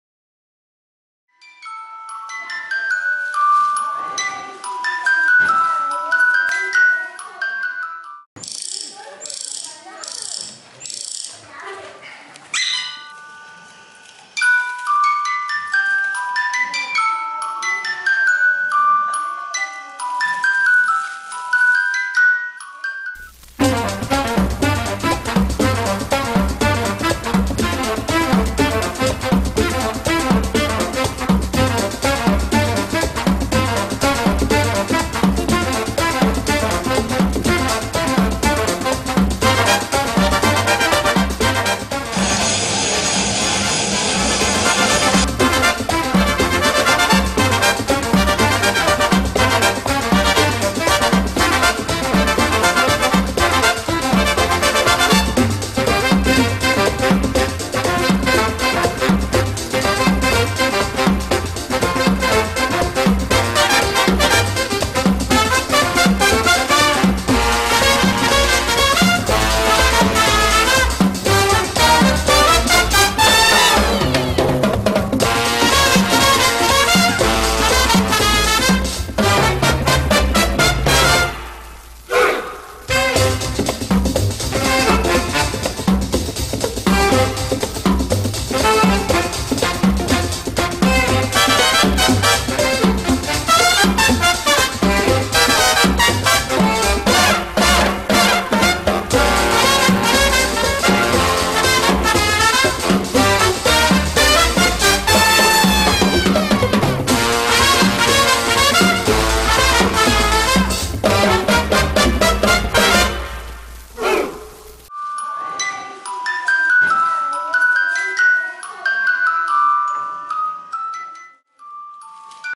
LA CAJA, video alcalá
Espectáculo de magia, pantomima y circo, para todos los públicos.